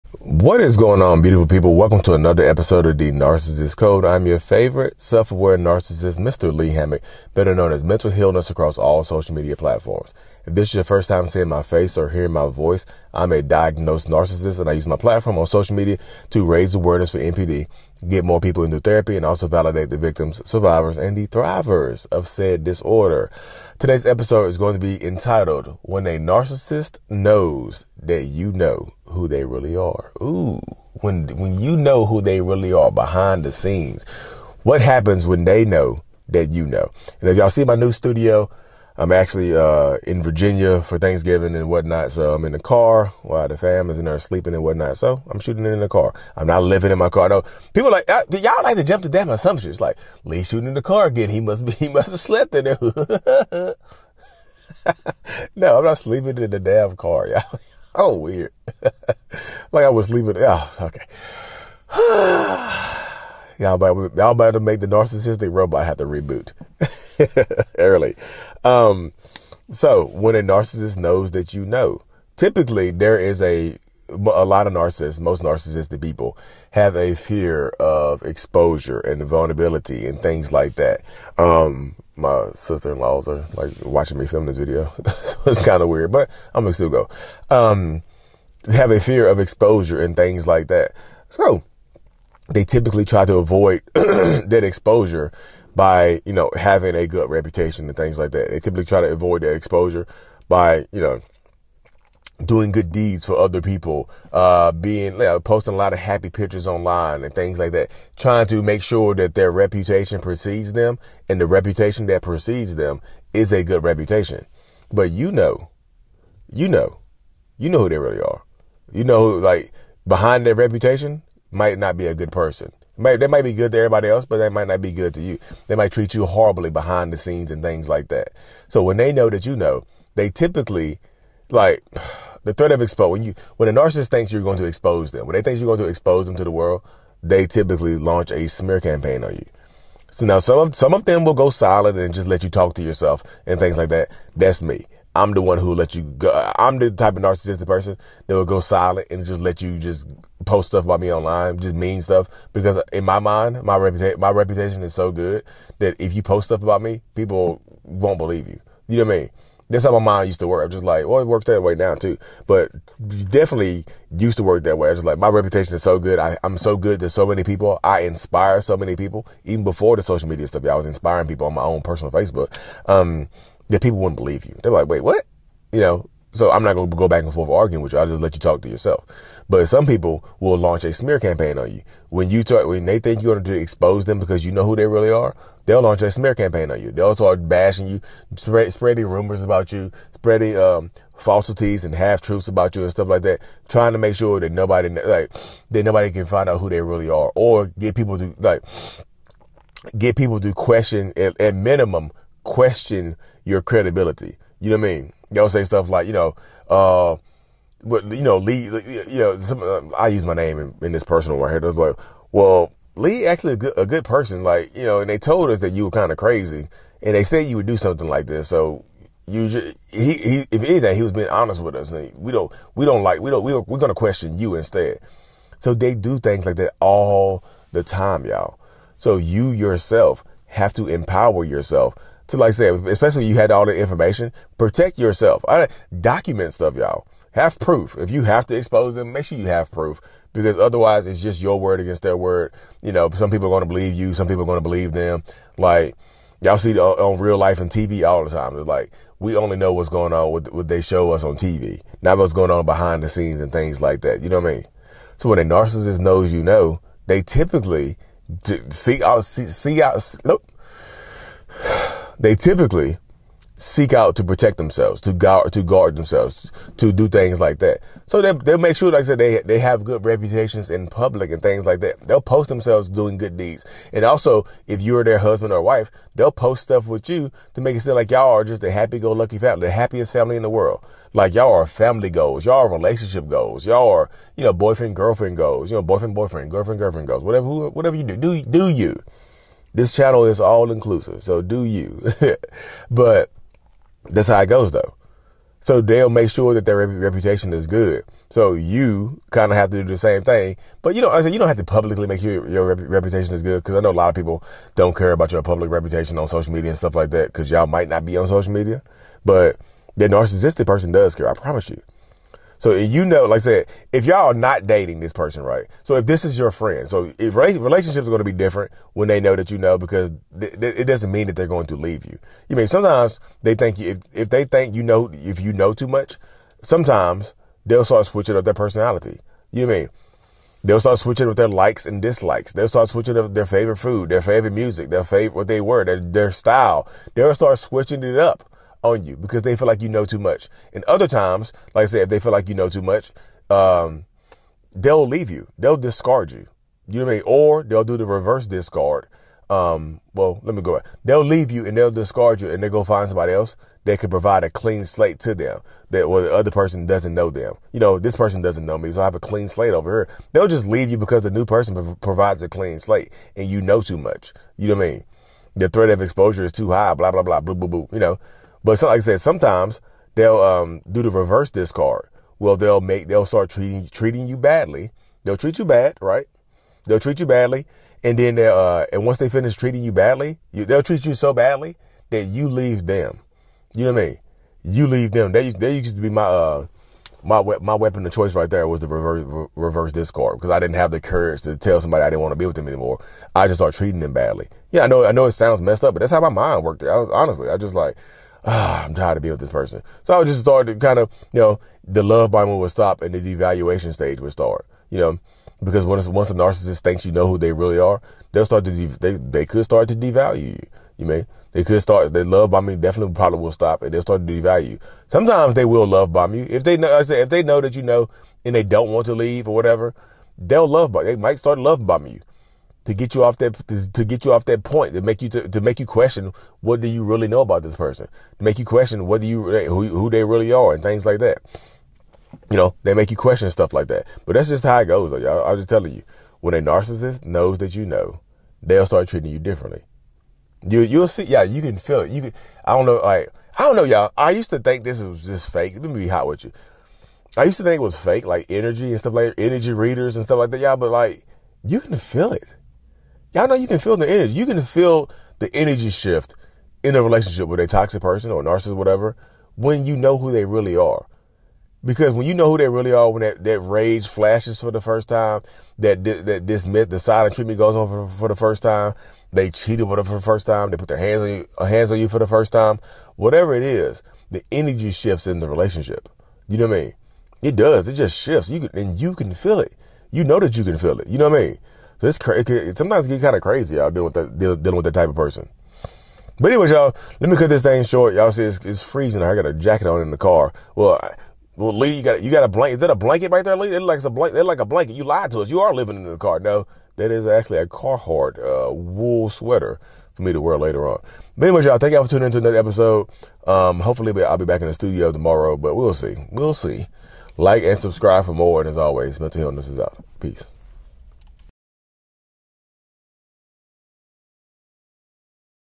When a Narcissists knows that you KNOW who they really are | The Narcissists' Code Episode 563
When a Narcissists knows that you KNOW who they really are and that you can expose them and ruin their reputation that they have worked hard to cultivate